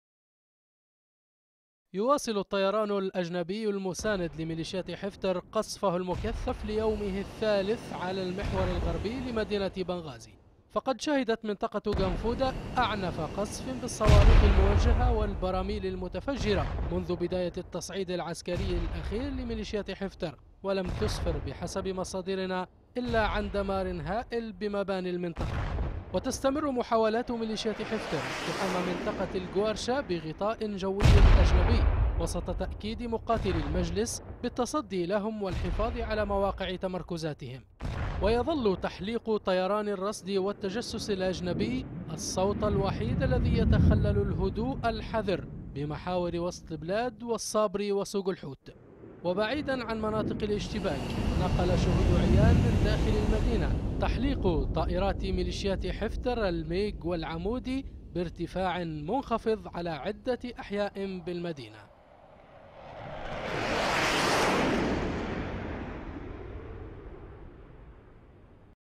الوضع الميداني و العسكري في مدينة بنغازي 28-07-2016
يواصل الطيران الأجنبي المساند لمليشيلت حفتر قصفه بالقدائف الموجهة و البراميل المتفجرة على مناطق المحور الغربي في بنغازي، و لازالت محاولات مليشيات حفتر التقدم نحو منطقة القوارشة مستمرة في ظل تصدي مقاتلي مجلس شورى ثوار بنغازي لهم.